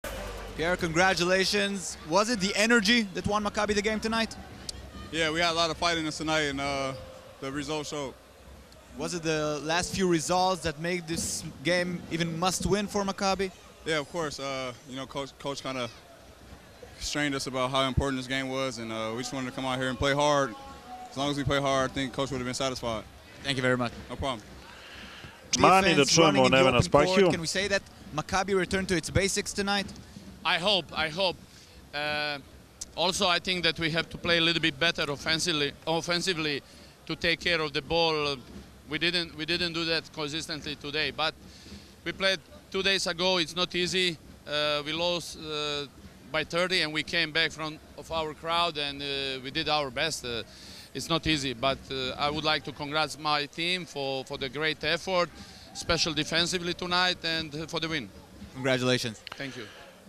Evroliga: Pjer Džekson i Neven Spahija Nakon Meča Maccabi - Crvena Zvezda | Sport Klub
Košarkaš izraelskog Makabija Pjer Džekson rekao je nakon pobede nad Crvenom Zvezdom u Evroligi da je njegov tim pokazao borbeni duh, što je bio glavni razlog za pobedu Makabija.

Govorio je i trener Makabija Neven Spahija, koji smatra da njegova ekipa mora da pobošlja igru u odbrani.